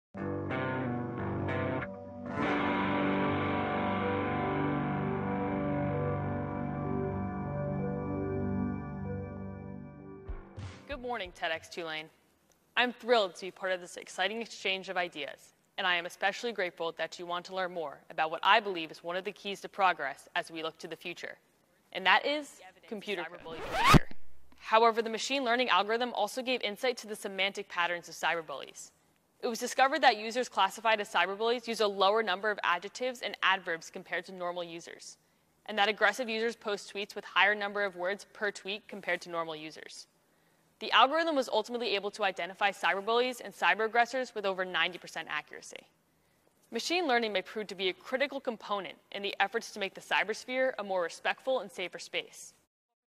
Data-mining the Sphere for Social Control +by+ DOXx4Sale
"... users classified as cyber-bullies, use a lower number of adjectives and adverbs compared to normal users..."

Controlled communications are deemed "normal" while intelligent autonomous communications are flagged.

"...aggressive users post tweets with higher number of words per tweet compared to normal users."